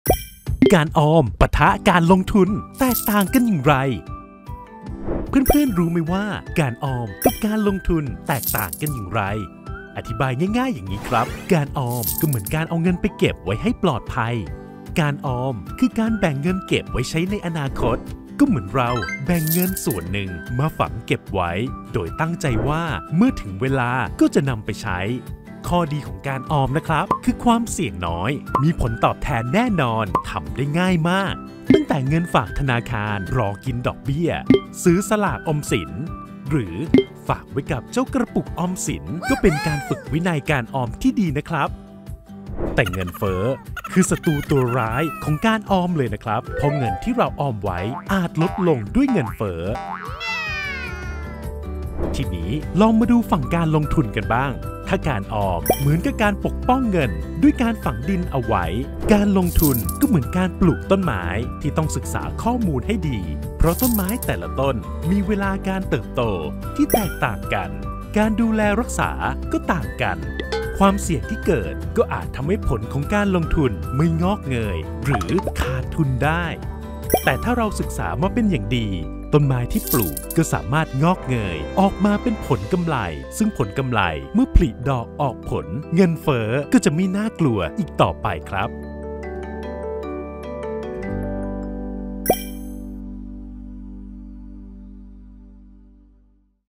การลงทุน | เรื่องที่ 24 การออม VS การลงทุน แตกต่างกันอย่างไร
มาทำความเข้าใจความหมายของการออมกับการลงทุนให้ลึกซึ้ง เพื่อจะได้รู้ว่าเราเหมาะกับการออมแบบไหนกัน
การออม ก็คือการแบ่งเงินไว้ใช้ในอนาคต ซึ่งข้อดีของการออมคือ ความเสี่ยงน้อย ได้ผลตอบแทนแน่นอน ทำได้ง่าย เช่น ออมแบบฝากเงินธนาคารเพื่อกินดอกเบี้ย ซื้อสลากออมสิน หรือหยอดกระปุกออมสิน
ส่วนการลงทุน ก็เหมือนการที่เราปลูกต้นไม้ ที่ต้นไม้แต่ละต้นมีเวลาในการเติบโตที่ต่างกัน การดูแลรักษาต่างกัน และความเสี่ยงนั้นก็ต่างกันด้วยเช่นกัน อาจจะงอกเงยขึ้นหรือขาดทุนก็สามารถเกิดขึ้นได้
ซึ่งทั้งสองวิธีนี้ เราสามารถเลือกสรรได้ตามที่ตัวเองชอบ จะออมแบบหยอดกระปุก ได้เงินที่แน่นอน หรือจะศึกษาวิธีการลงทุนในรูปแบบต่างๆ ก็ได้นะ